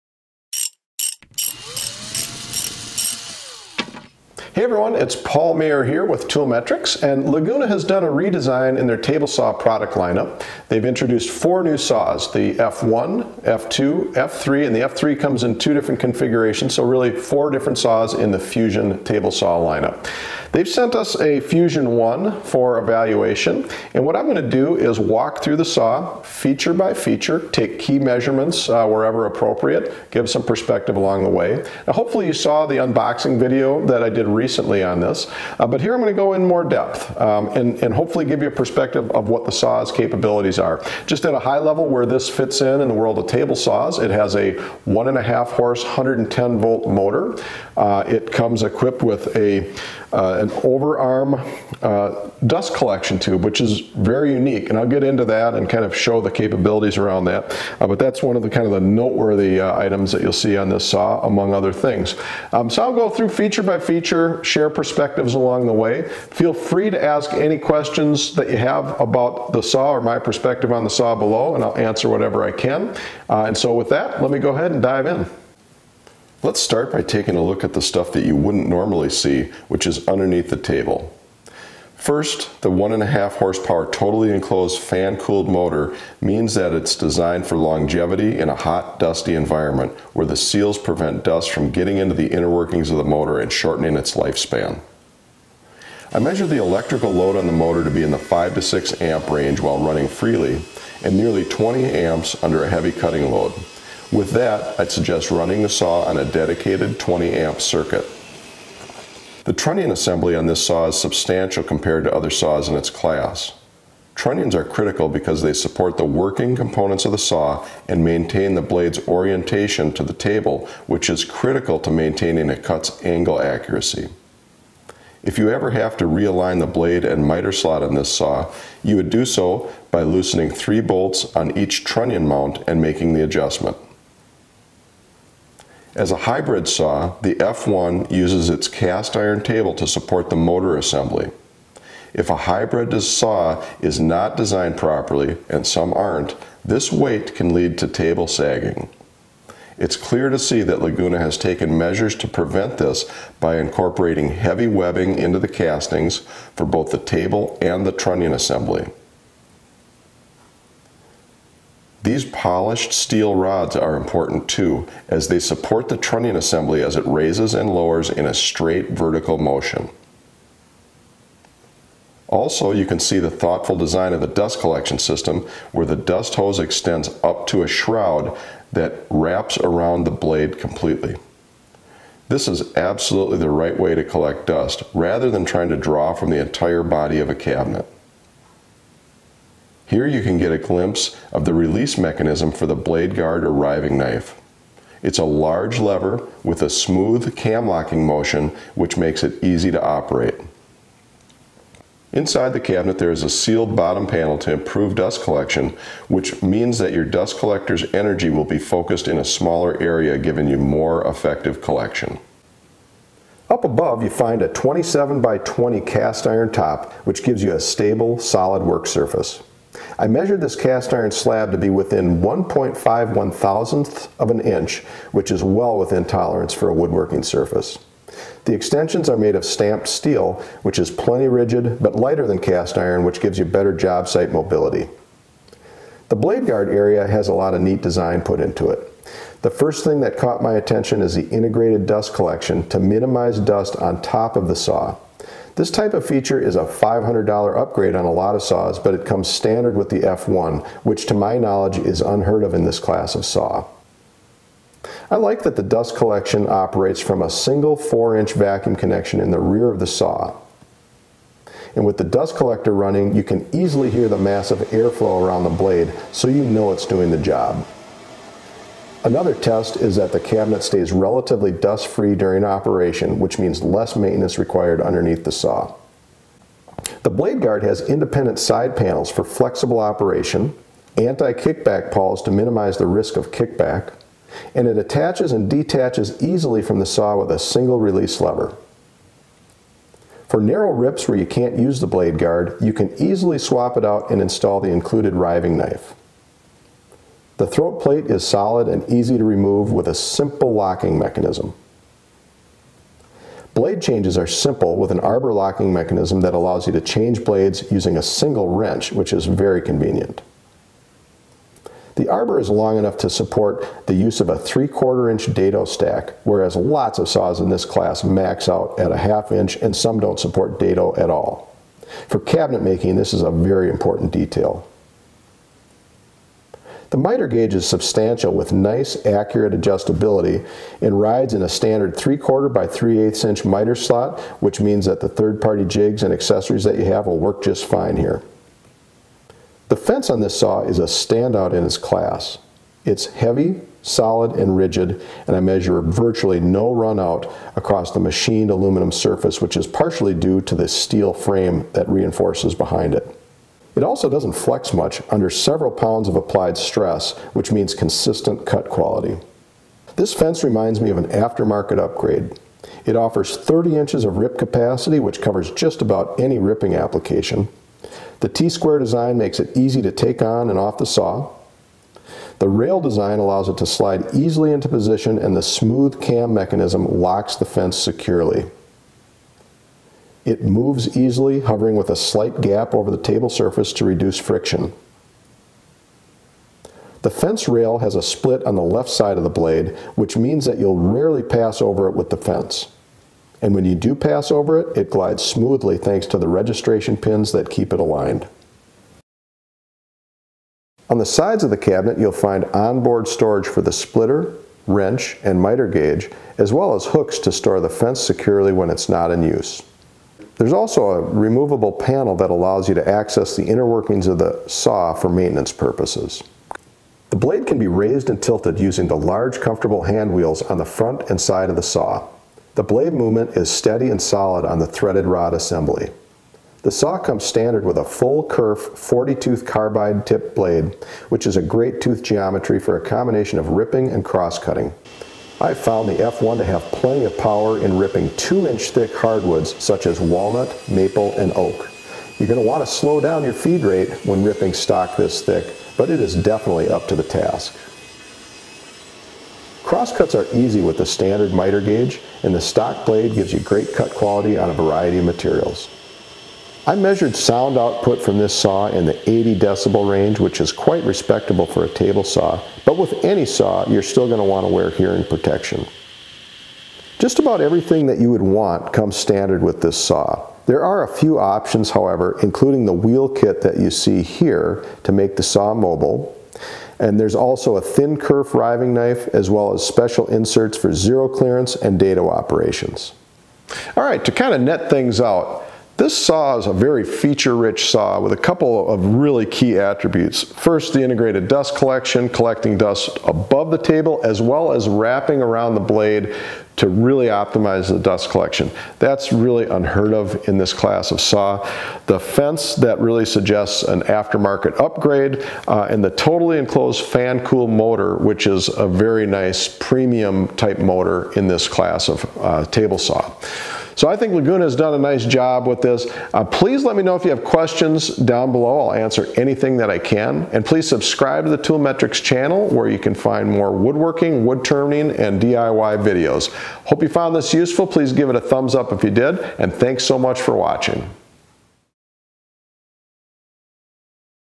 Laguna Fusion F1 Table Saw Deep Dive Examination
Laguna has recently introduced a new hybrid table saw; the Fusion F1.  Laguna provided a demo unit of this saw for evaluation purposes.  The table saw has a lot of cool features like a fully integrated, standard above table dust collection system, 1.5 HP TEFC motor, magnetic switch, and enclosed stand.  Here I do a deep dive examination of the Fusion F1 table saw, examining all key features and putting the Fusion F1 Table saw through its paces.